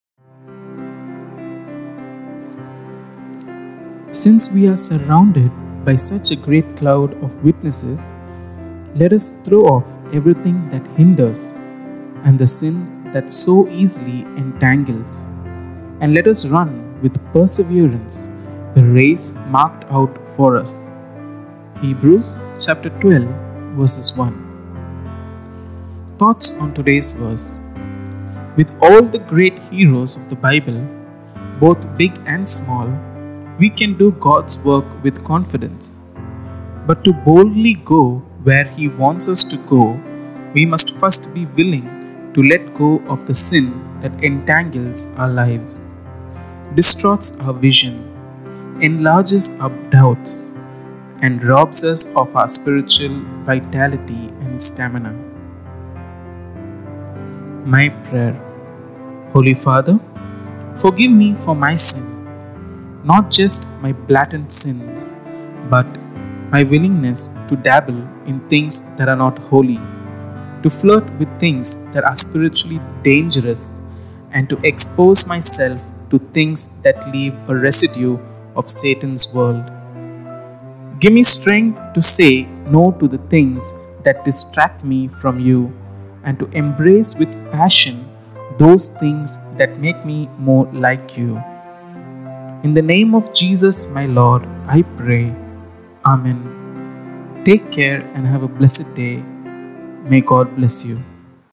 Verse And Prayer For The Day | 27th July 2022 | Bible Verse | My Life With God
Since we are surrounded by such a great cloud of witnesses, let us throw off everything that hinders and the sin that so easily entangles, and let us run with perseverance the race marked out for us.
—Hebrews 12:1

With all the great heroes of the Bible, both big and small, we can do God's work with confidence. But to boldly go where he wants us to go, we must first be willing to let go of the sin that entangles our lives, distorts our vision, enlarges our doubts, and robs us of our spiritual vitality and stamina.

My Prayer...

Holy Father, forgive me for my sin. Not just my blatant sins, but my willingness to dabble in things that are not holy, to flirt with things that are spiritually dangerous, and to expose myself to things that leave a residue of Satan's world. Give me strength to say no to the things that distract me from you and to embrace with passion those things that make me more like you. In the name of Jesus my Lord I pray. Amen.